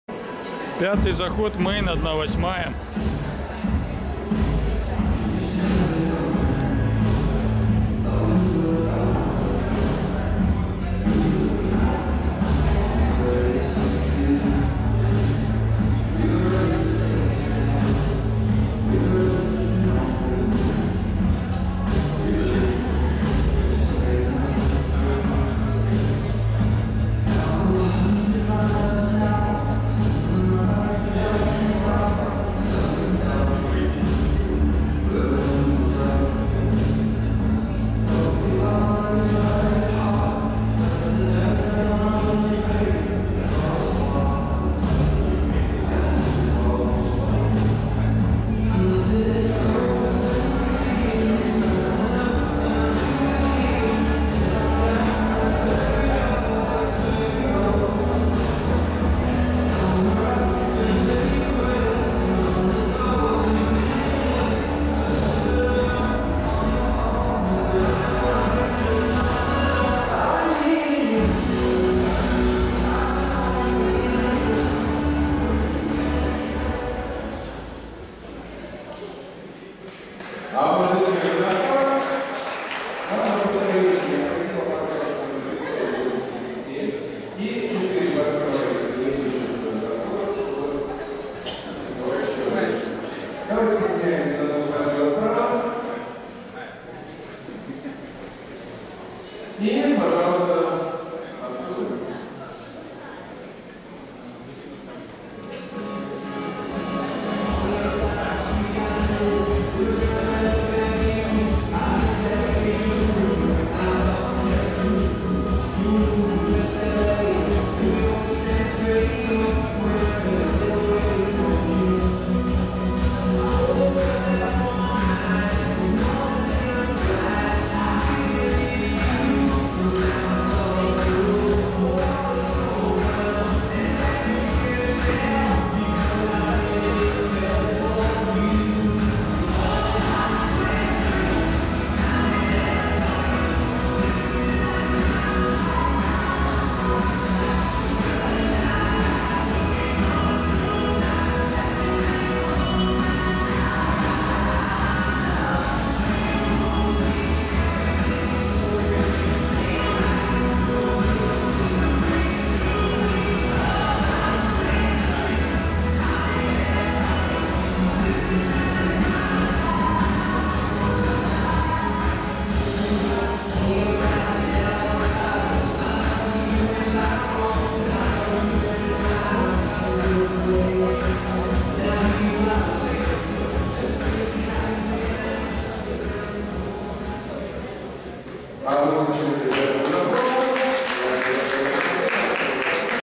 24.11.18 Надежды России 1/8 J&J Main заход 5
Надежды России - рейтинговый турнир АСХ
состоялся: 24 ноября 2018 года
место проведения: ТЦ «Пингвин» (м. Крымская МЦК, м. Академическая) - Севастопольский Проспект 11А1
Непосредственные организаторы: Кудрявцев Михаил, Кудрявцева Ирина
Видео: Игорь Скобелев